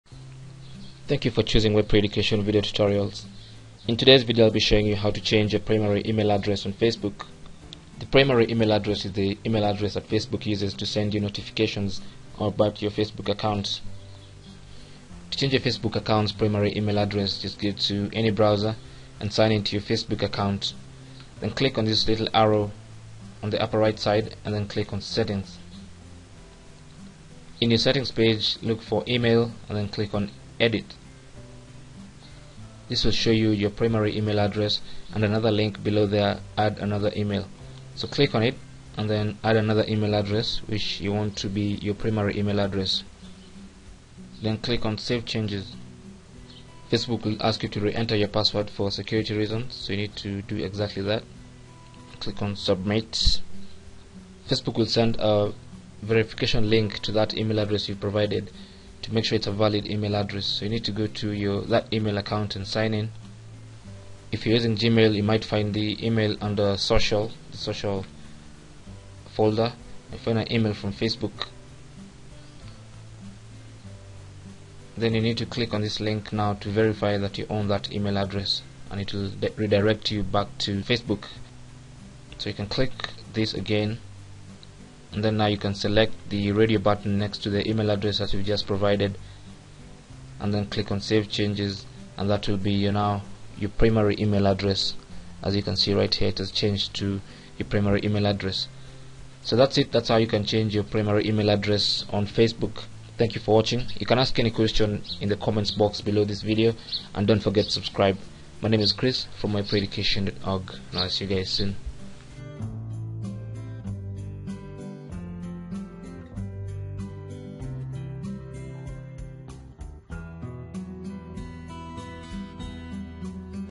How To Change Facebook Email Address
This is the address Facebook uses to send notifications to you.
Go to Settings in your facebook account.
Click Edit under Email
Add another Email
Click Save changes.